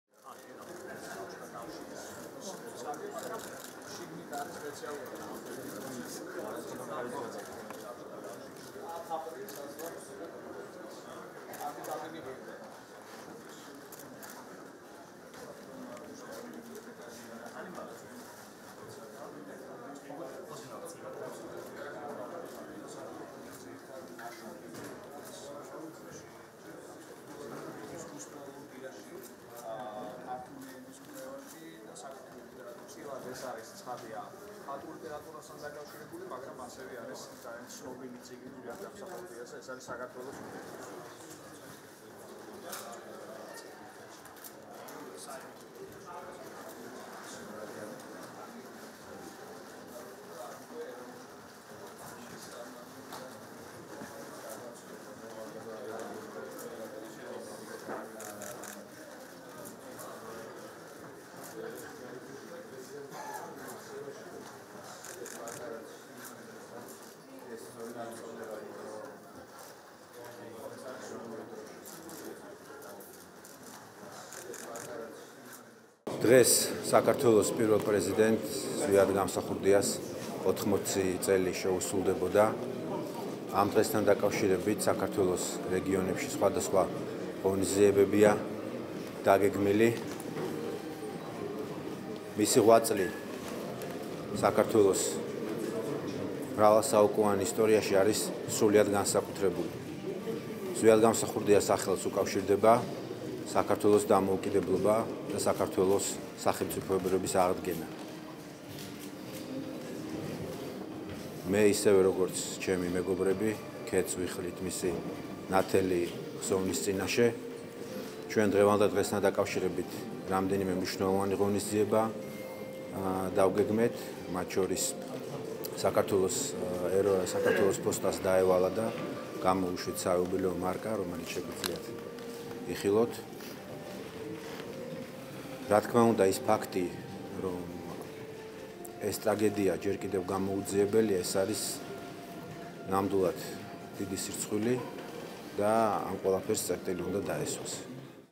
ზვიად გამსახურდიას 80 წლის იუბილეს აღსანიშნავი გამოფენა
ზვიად გამსახურდიას სახელს უკავშირდება საქართველოს დამოუკიდებლობა და საქართველოს სახელმწიფოებრიობის აღდგენა. მე, ისევე როგორც ჩემი მეგობრები, ქედს ვიხრით მისი ნათელი ხსოვნის წინაშე, - ამის შესახებ, საქართველოს პრემიერ-მინისტრმა მამუკა ბახტაძემ, საქართველოს პირველი პრეზიდენტის ზვიად გამსახურდიას 80 წლის იუბილეს აღსანიშნავად გამართულ გამოფენაზე განაცხადა.

პირველი პრეზიდენტის იუბილესთან დაკავშირებით ეროვნულ ბიბლიოთეკაში ზვიად გამსახურდიას ფოტოები, თარგმანები და პირველი პრეზიდენტის შესახებ გამოცემული წიგნები გამოიფინა.

„დღეს, საქართველოს პირველ პრეზიდენტს, ზვიად გამსახურდიას 80 წელი შეუსრულდებოდა. ამ დღესთან დაკავშირებით, საქართველოს რეგიონებში სხვადასხვა ღონისძიებებია დაგეგმილი. მისი ღვაწლი საქართველოს მრავალსაუკუნოვან ისტორიაში არის სრულიად განსაკუთრებული. საქართველოს ფოსტას დაევალა და გამოვუშვით საიუბილეო მარკა. რა თქმა უნდა, ფაქტი, რომ ეს ტრაგედია ჯერ კიდევ გამოუძიებელია, ეს არის ნამდვილად დიდი სირცხვილი და ამ ყველაფერს წერტილი უნდა დაესვას", - განაცხადა მამუკა ბახტაძემ.

გამოფენაზე წარმოდგენილია ასევე ზვიად გამსახურდიას ხუთტომეული, სერიიდან „საქართველოს სახელმწიფოს ხელმძღვანელები". გაიმართა ასევე საფოსტო მარკის წარდგენა, რომელიც საქართველოს ფოსტამ ზვიად გამსახურდიას 80 წლის იუბილეს აღსანიშნავად გამოსცა.

პრემიერ-მინისტრმა გამოფენა საქართველოს პარლამენტის თავმჯდომარესთან და თბილისის მერთან ერთად დაათვალიერა.